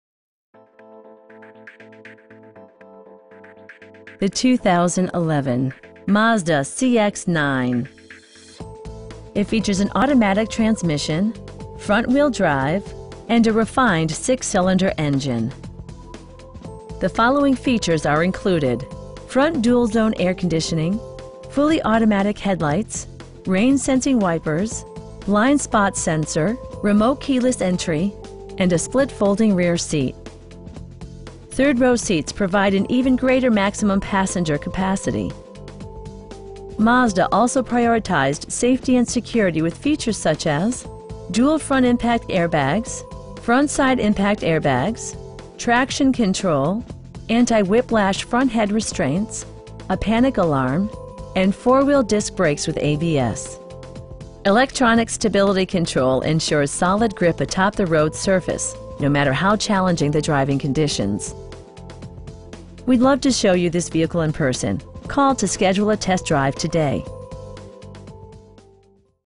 2011 Mazda CX-9 Grand Touring in Oklahoma City, OK 73131
Bob Moore Mazda
13045 N. Kelley

The 2011 Mazda CX-9. It features an automatic transmission, front-wheel drive, and a refined 6 cylinder engine. The following features are included: front dual-zone air conditioning, fully automatic headlights, rain sensing wipers, blind spot sensor, remote keyless entry, cruise control, and a split folding rear seat. Third row seats provide an even greater maximum passenger capacity. Mazda also prioritized safety and security with features such as: dual front impact airbags, front SIDE impact airbags, traction control, anti-whiplash front head restraints, a panic alarm, and 4 wheel disc brakes with ABS. Electronic stability control ensures solid grip atop the road surface, no matter how challenging the driving conditions. We'd love to show you this vehicle in person.